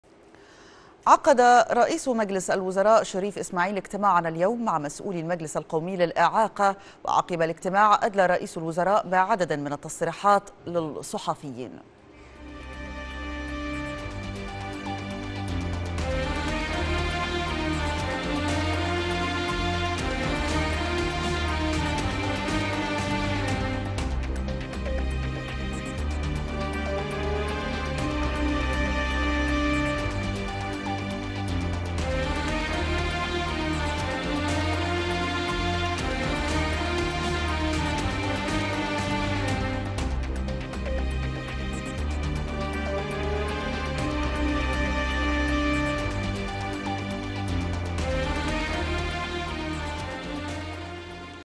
رئيس الوزراء يرأس اجتماعا للمجلس القومى للإعاقة
الموقع الرسمي للقناة  ..